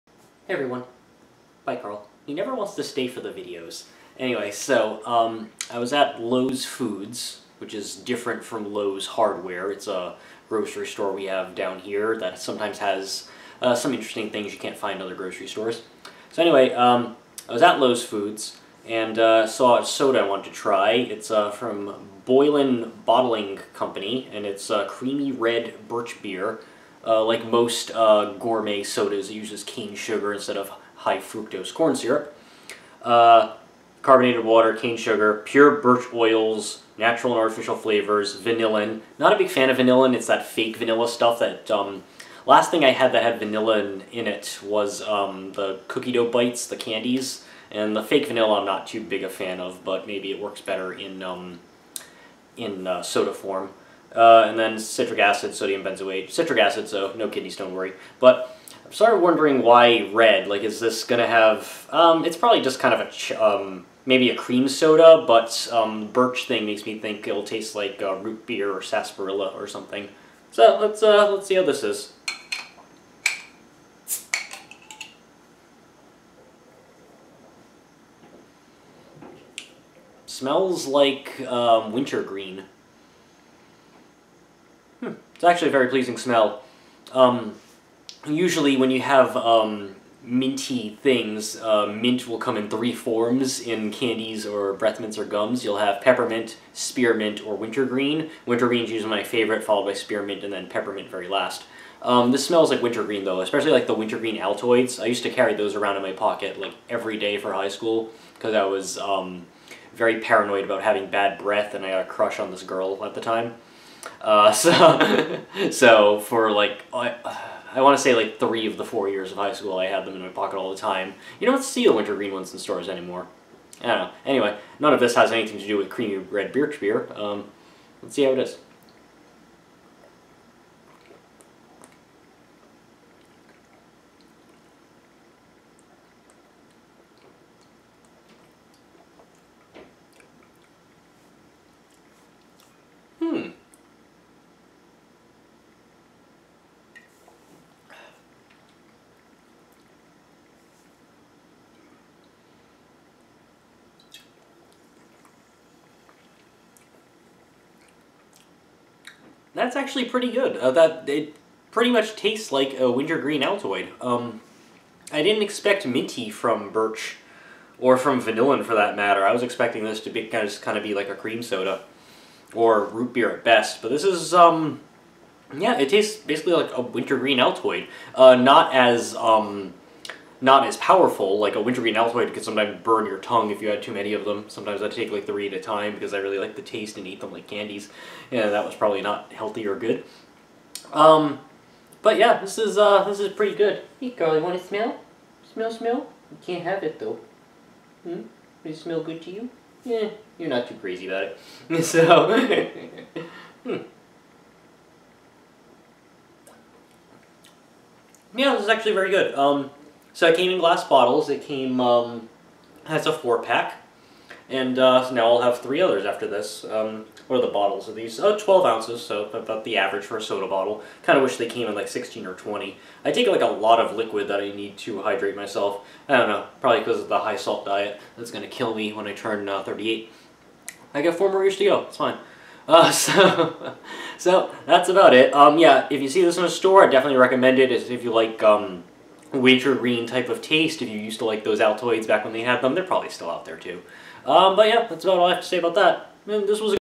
Leninhawk's Trying: Creamy Red Birch Beer by Boylan Bottling Co.
Saw this soda and was intrigued. So here you go!